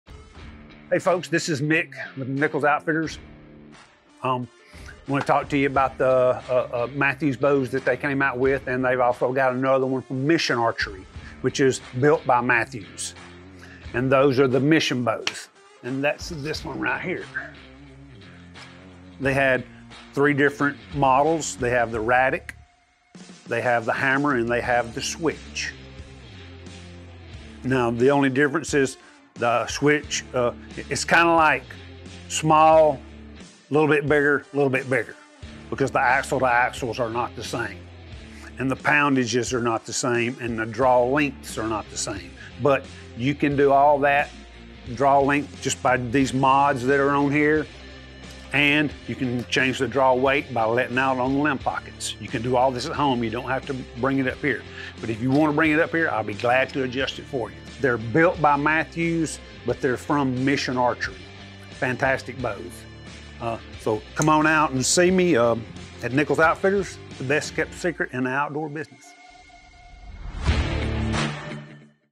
Nichols Outfitters Reviews the Latest Mission Bows Lineup
Here's Mick highlighting the latest from Mission Bows by Mathews. As an industry leader in innovation and performance, Mission Archery has once again outdone themselves, delivering a diverse range of bows to cater to every type of archer.

We carry the RADIK, HAMMR, and the SWITCH models. Each with slight variations.  Stay ahead of the curve and enhance your archery experience with the latest Mission Bows.